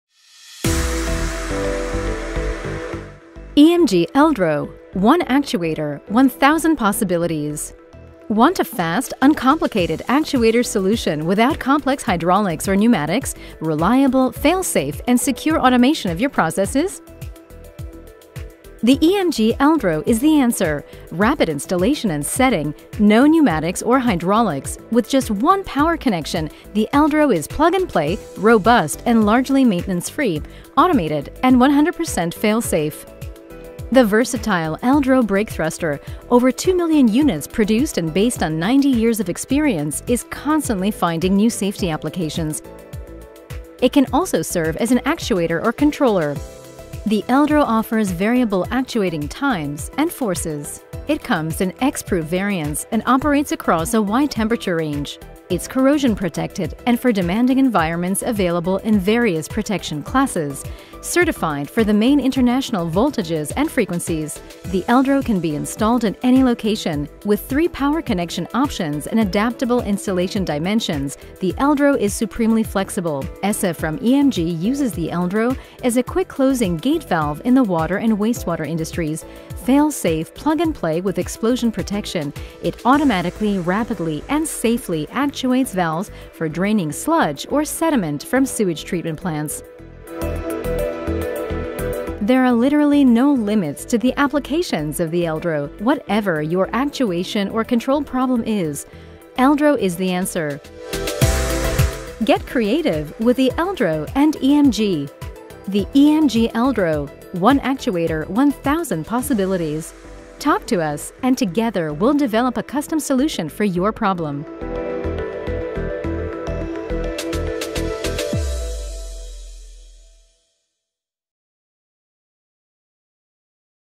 EMG ELDRO® / ELHY® and EMG ESSE - Electro hydraulic actuators
EMG ELDRO® / ELHY® Area of application - In addition to a wide range of applications in the mechanical engineering industry, materials lifting and handling can be considered as the main uses of industrial brakes offered on the market.

EMG ESSE – Quick Response Actuator for wastewater treatment plant or individual applications.

EMG generally use in mining, steel, port, and building industry applications.

System solutions and products for cranes, electro-hydraulics, handling technology, industrial applications, ships, trains and construction machines.